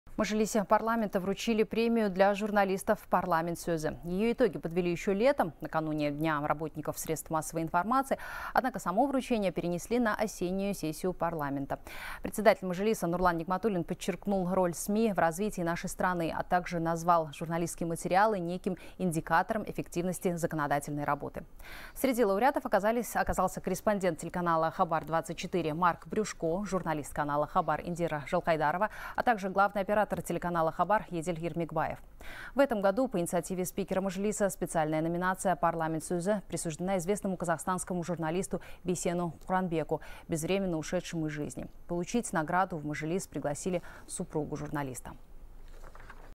Сотрудники Агентства «Хабар» получили премию «Парламент сөзі»
В Мажилисе Парламента вручили Премию для журналистов «Парламент сөзі», сообщает корреспондент «Хабар 24».

Её итоги подвели ещё летом накануне Дня работников средств массовой информации. Однако само вручение перенесли на осеннюю парламентскую сессию. Председатель Мажилиса Нурлан Нигматулин подчеркнул роль СМИ в развитии нашей страны, а так же назвал журналистские материалы неким индикатором эффективности законодательной работы.